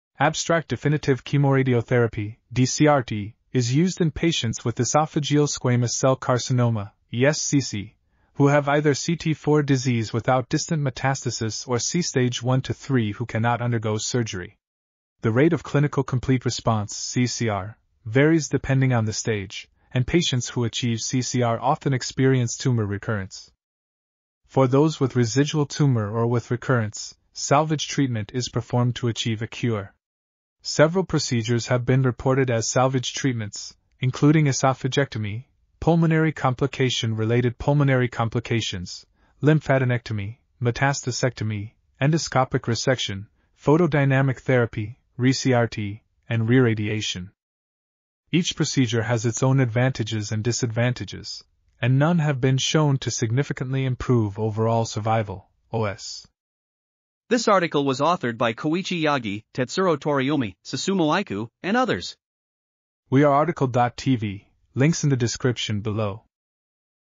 Salvage treatment after definitive chemoradiotherapy for esophageal squamous cell car... | RTCL.TV
### Keywords ###

### Article Attribution ###
Title: Salvage treatment after definitive chemoradiotherapy for esophageal squamous cell carcinoma
Authors: Koichi Yagi, Tetsuro Toriumi, Susumu Aikou, Hiroharu Yamashita ,and Yasuyuki Seto
Publisher: Wiley
DOI: 10.1002/ags3.12448

### Video Timestamps ###
0:00:00 - Summary
0:00:57 - Title
0:01:02 - Outro
0:01:07 - End